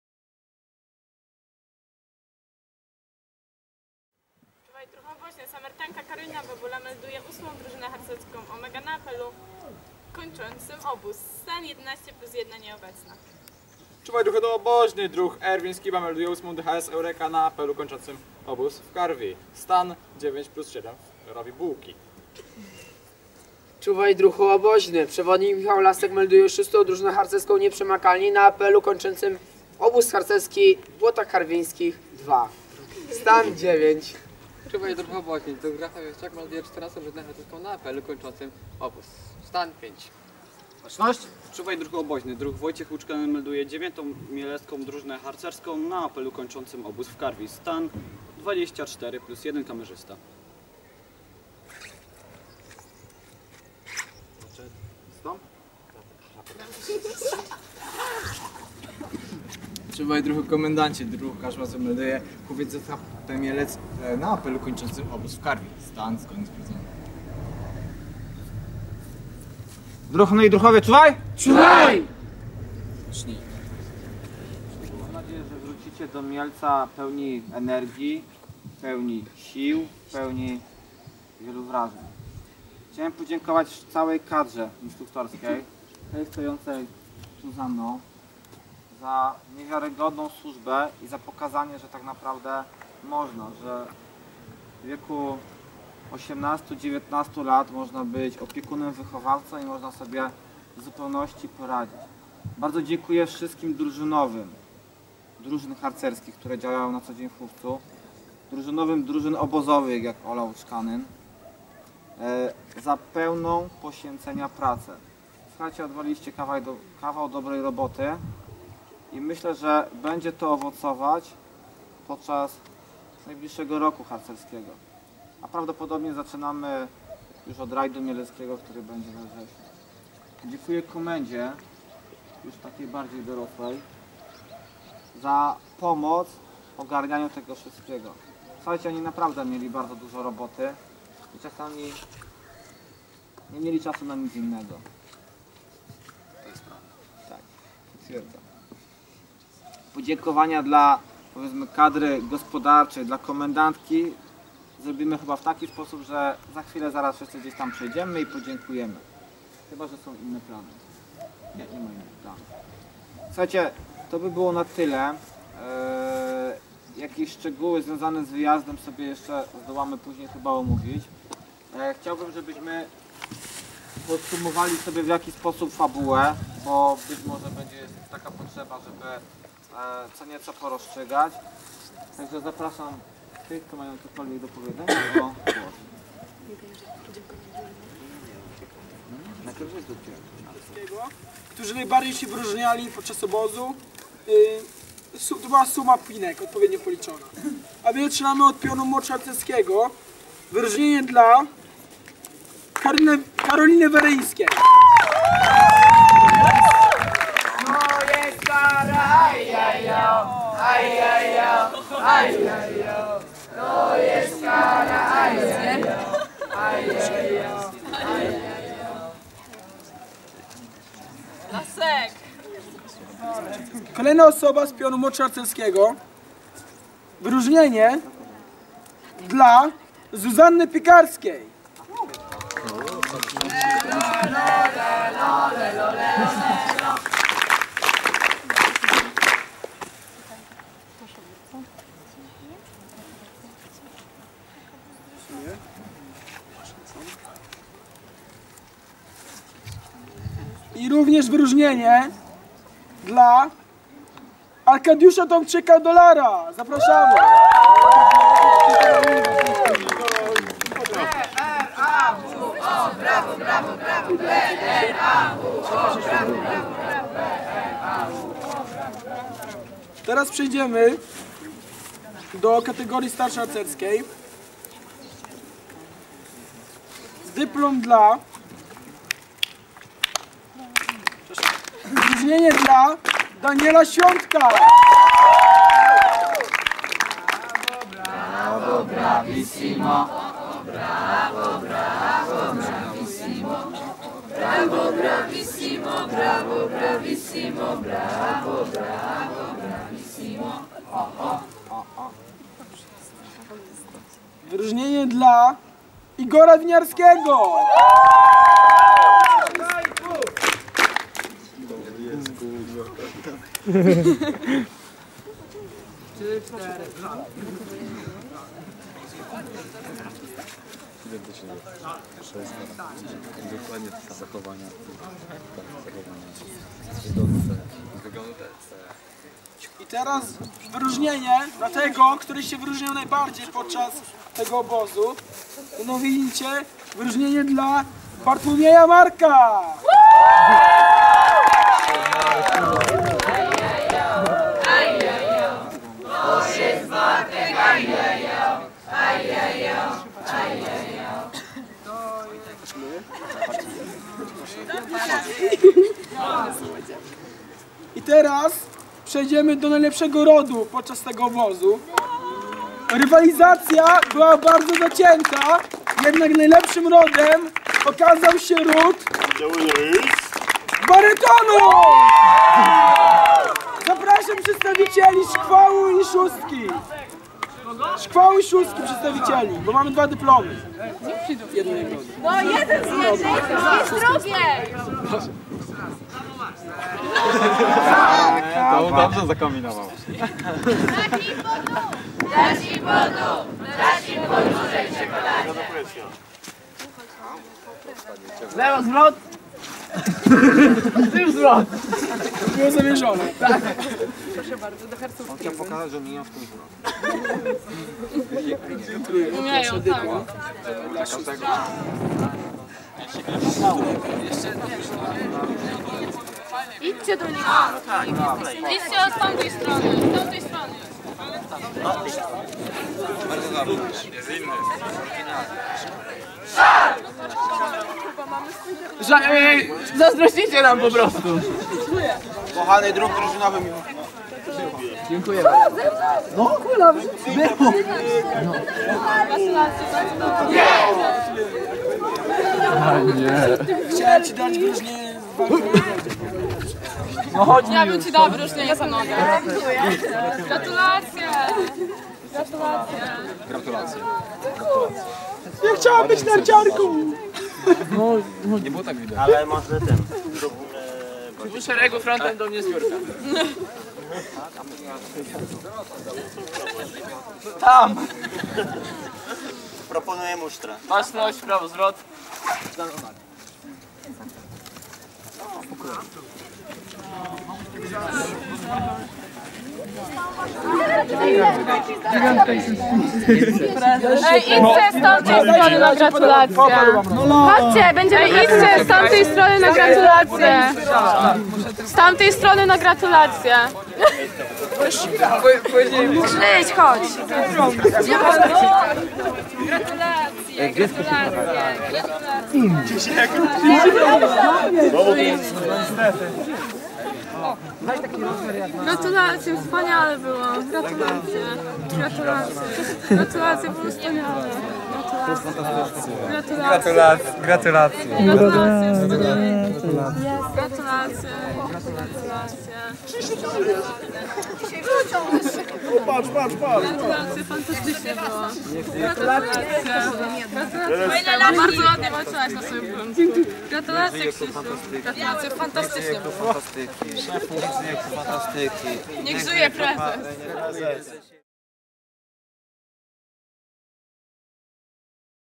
Obóz Harcerski Hufca ZHP Mielec Karwia 2012 - apel końcowy sobota 14 lipca 2012r. część 1
Obóz Harcerski Hufca ZHP Mielec
Karwia 2012
Baza obozowa Hufca ZHP Mielec
''Słoneczna Polana''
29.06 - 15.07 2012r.

apel końcowy
sobota 14 lipca 2012r.
część 1